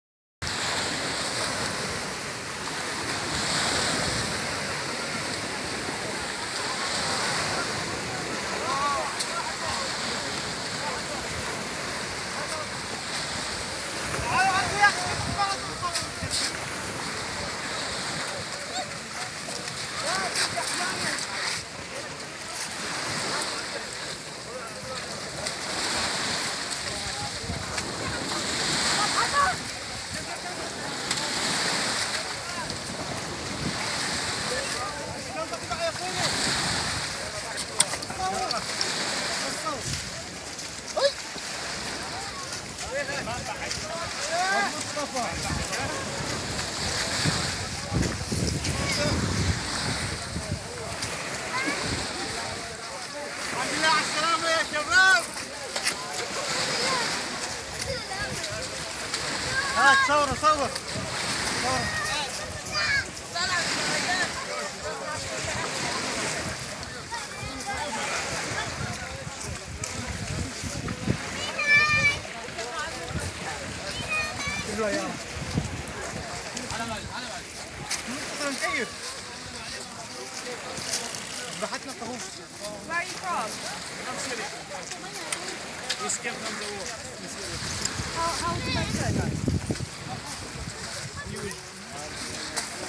Συριοι μετανάστες στη Κω
Αφιξη Συριων μεταναστών στα παραλια της Κω